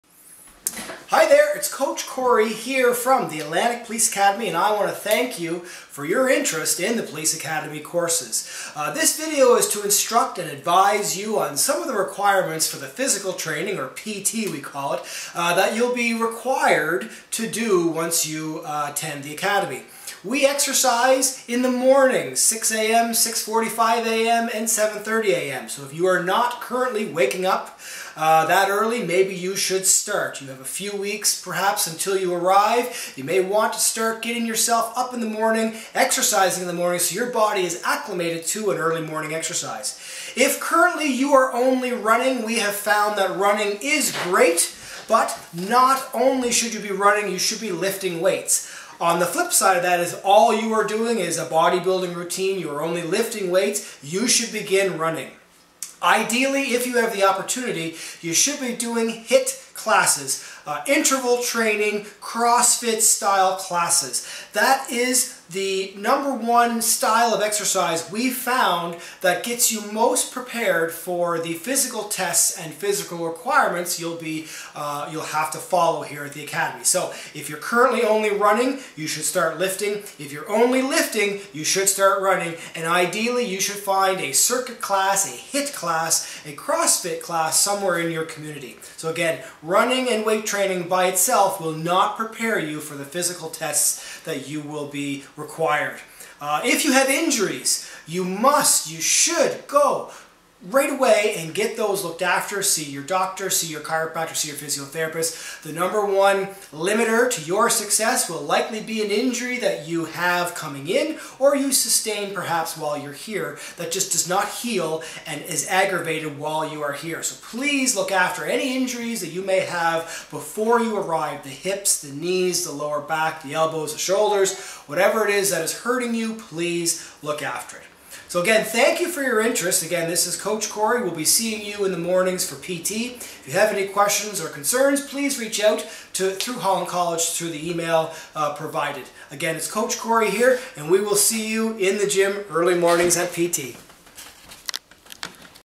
Physical Training Requirements, Tips & Best Practices for the Atlantic Police Academy.
Coach Corey Details the Physical Training Requirements, Tips & Best Practices for your success at the Atlantic Police Academy.